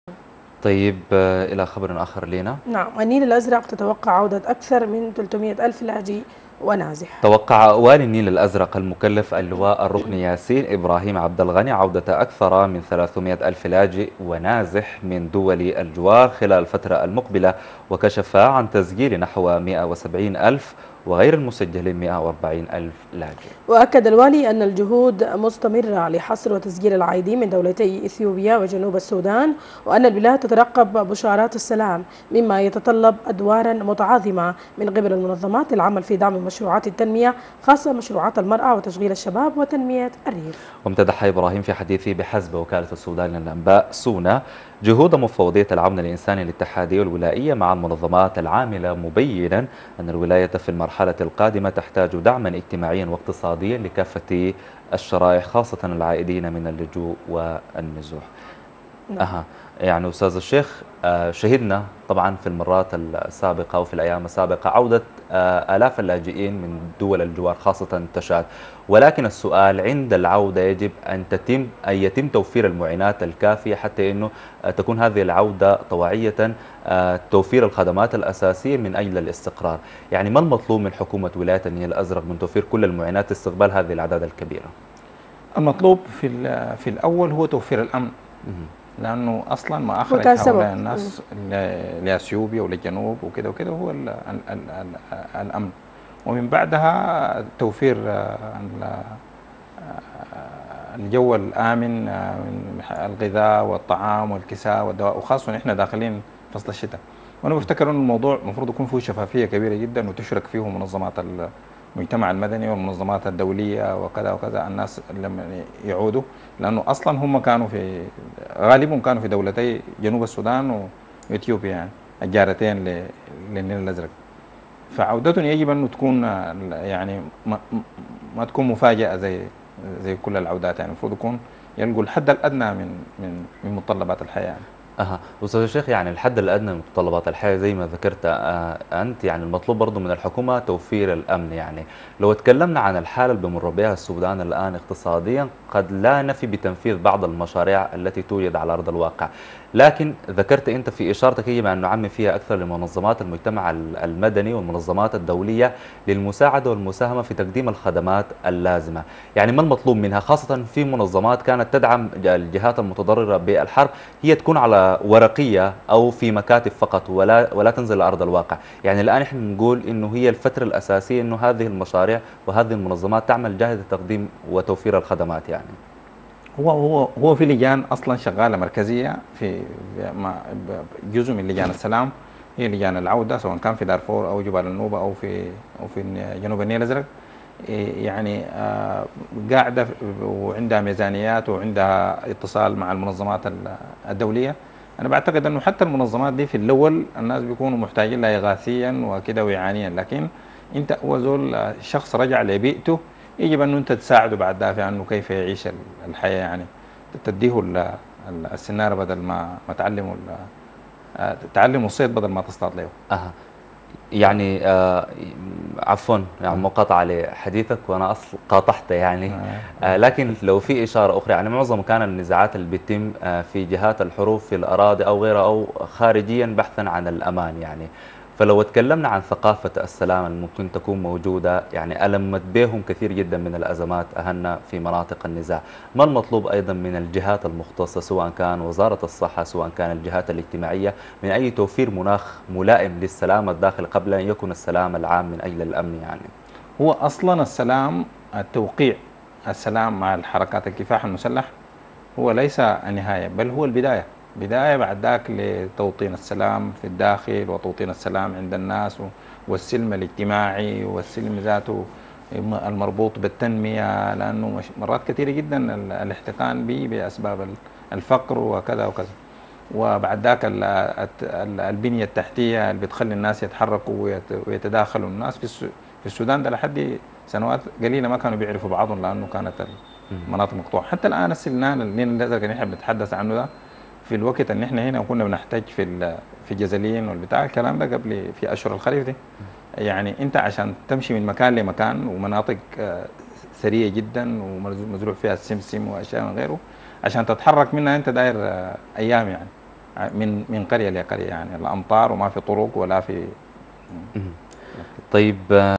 النيل الأزرق تتوقع عودة أكثر من 300 ألف لاجئ ونازح
الشروق نت | النيل الأزرق تتوقع عودة أكثر من 300 ألف لاجئ ونازح
أستاذ الشيخ يوسف ـ رئيس تحرير شبكة الشروق نت يحلل هذا الخبر
ـــــــــــــــــــــــــــــــــــــــــــــــــــــــــــــ

الشروق_ الفضائية
Ashorooq Tv
ـــــــــــــــــــــــــــــــــــــــــــــــــــــــــــــ